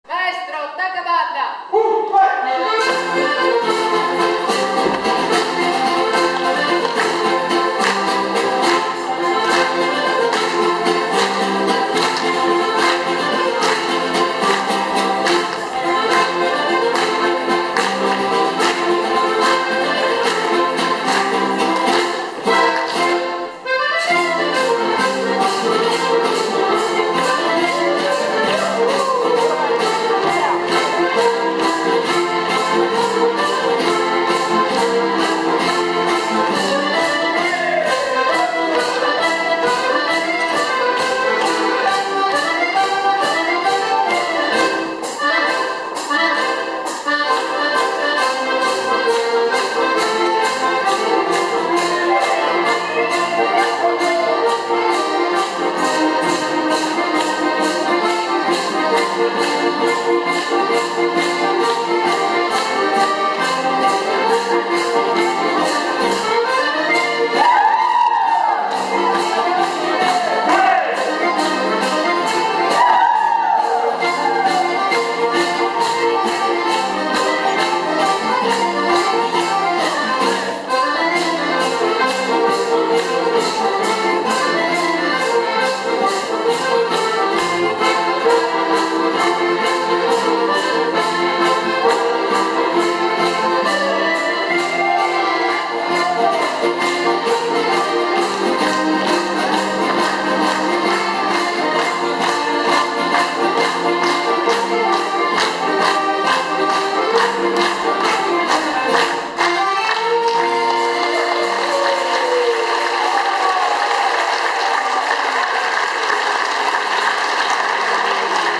Gruppo Folcloristico Trevigiano
Radomsko znalazło się na trasie koncertowej XXX Międzynarodowych Warsztatów Folklorystycznych.